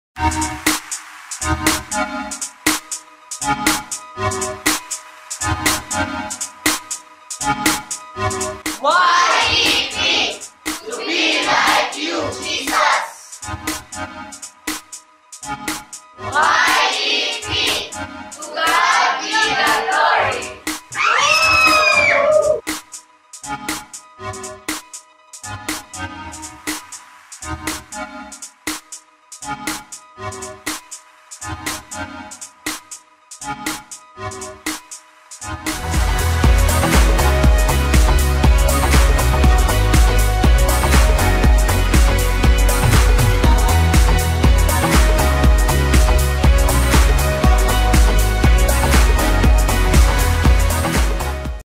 zBREAKAWAY 19 YEP (Youth Empowerment Program) Talk: DISCIPLESHIP ❤
Thank you YEP (Youth Empowerment Program) of ACEIPI (Advocates for Children and Elders International Philippines Incorporated) for having me as your speaker on Discipleship. To treat others like Jesus, to be like Jesus, and to let the light of God shine through you, you become a disciple of Jesus. Follow Jesus and be Jesus to others. Do it out of love and love of others. And as you live everyday, always keep those you learned in your heart.  ❤ May you be blessed more and may all your dreams come true. May your passion burn brightly so that in God's time, you will set the world on fire.❤

In God's time, your passion will set the world on fire ❤
Be the blessing ❤
And your dreams will come true ❤ =)